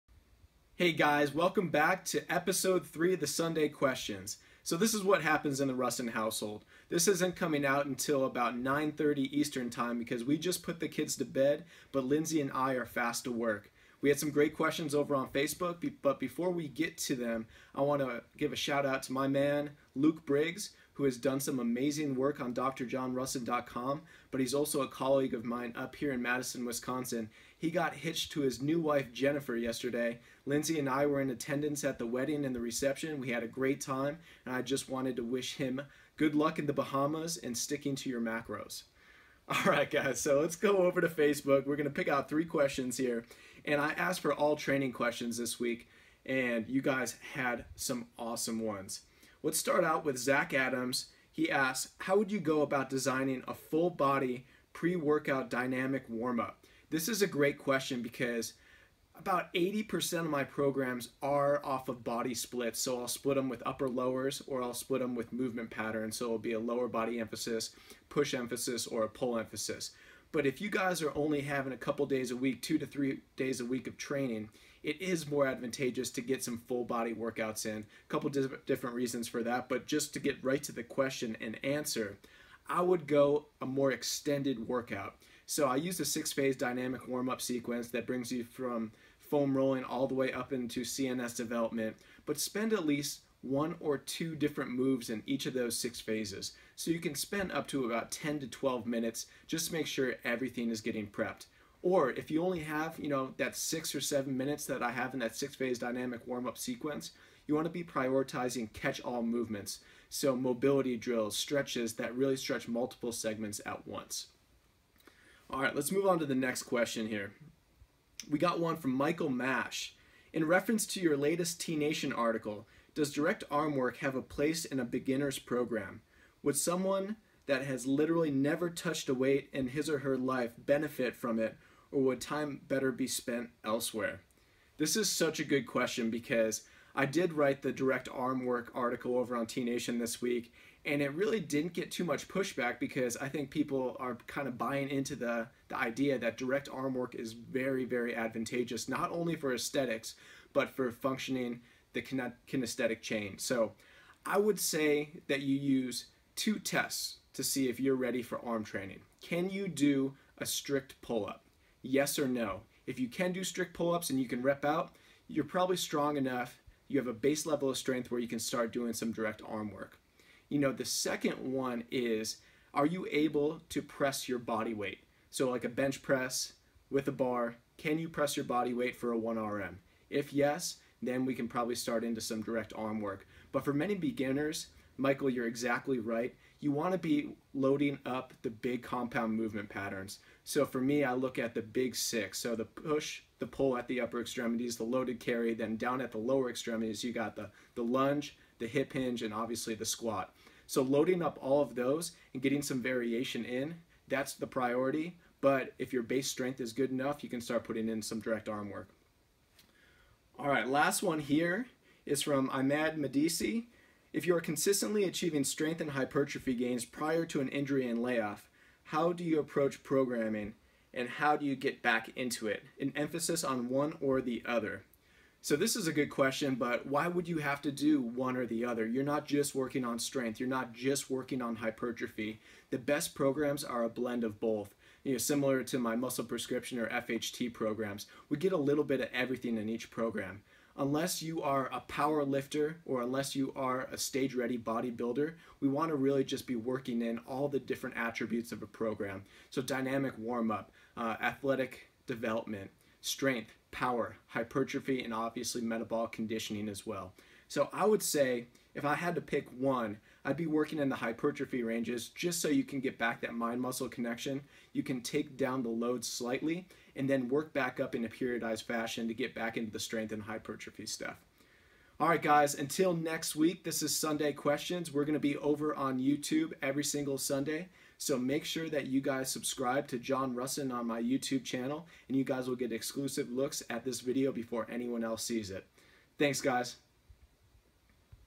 Sunday Questions Episode 3
In this episode of Sunday Questions, Dr. John Rusin reviews what type of training to focus on when coming back from an injury, the need for direct arm training for novice lifters and how to design the perfect dynamic warm up for a full body lifting session.